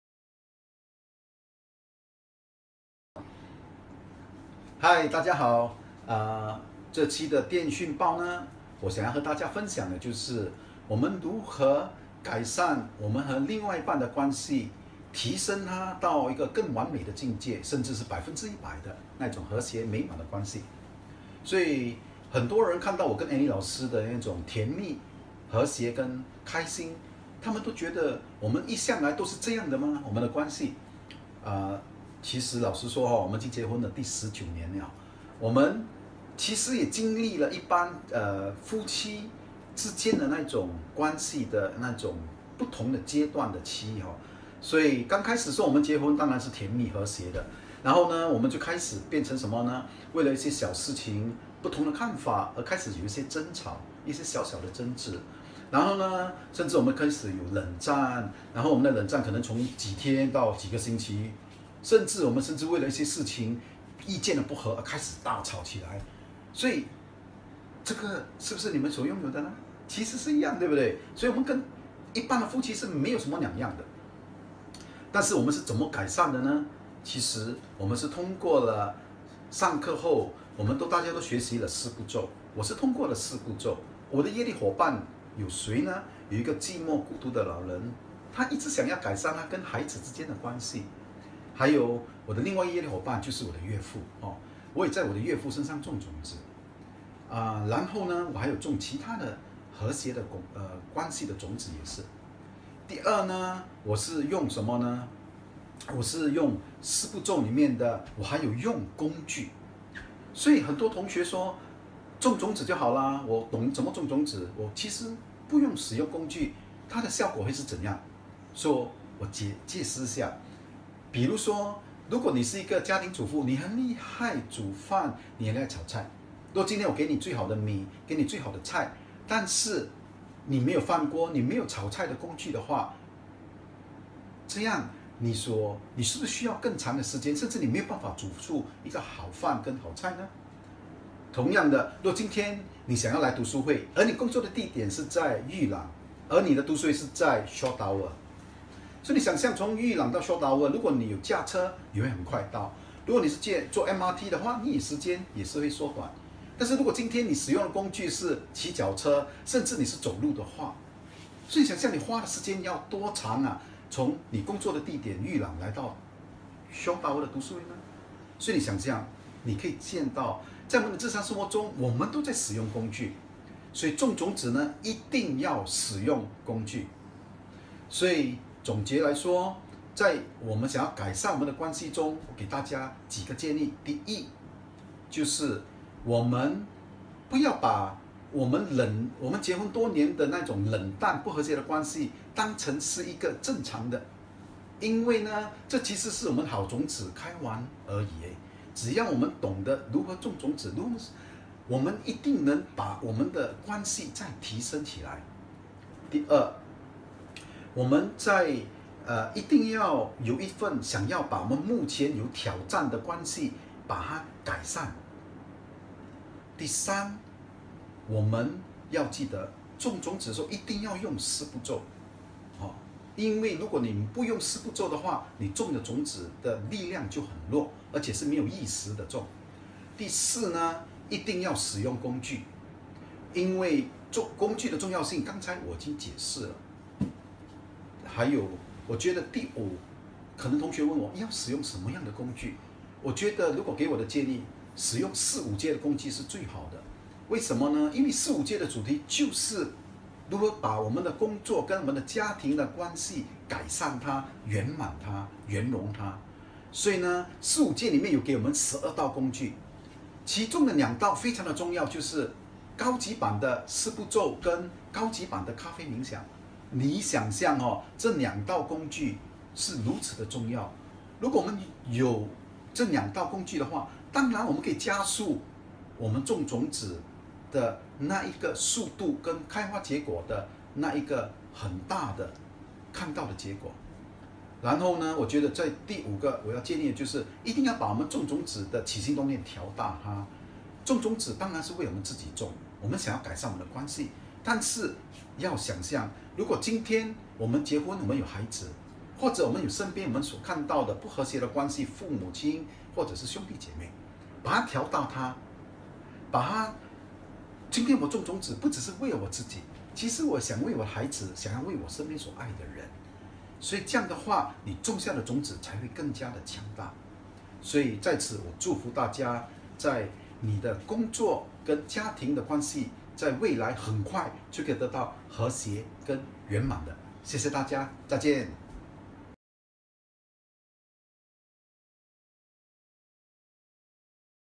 十一月份电讯报 | 如何改善和提升你的伴侣 | DCI Sunny老师与你分享 | 金刚商法
与你另外一半的关系搞到僵硬？那么到底要怎样的种出以及提升另外一半的关系。让sunny 老师分享给你们听！

如想要购买麦克.罗区格西的书, 请联络以下:
Please contact below information to purchase book:
100 Beach Rd, Shaw Towers, #02-41
Singapore 189702